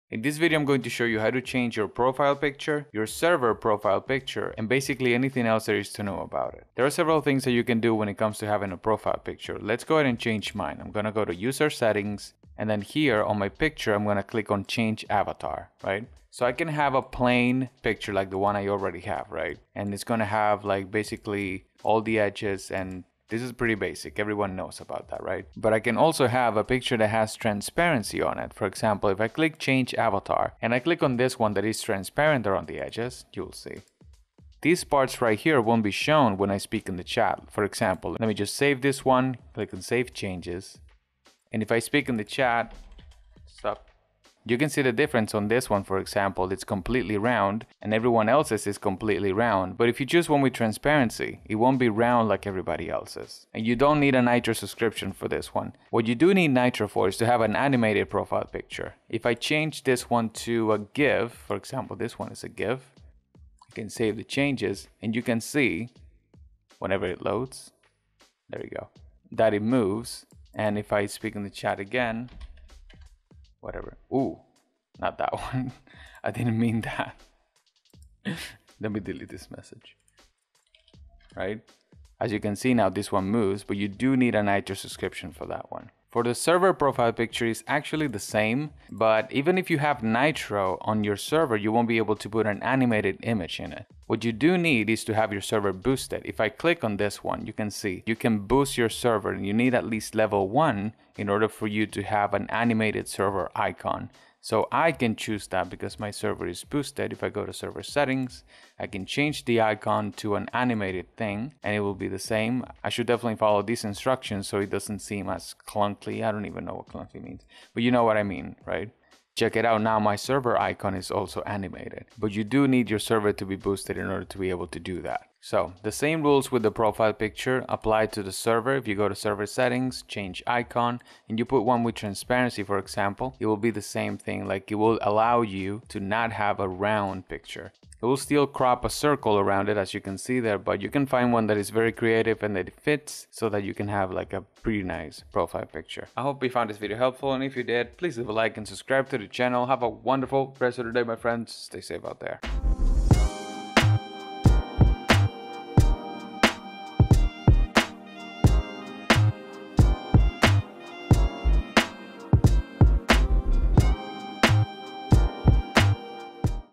How to Change Server Image & Profile Picture on Discord
In this video, I show you everything there is to know about profile pictures and server images on Discord.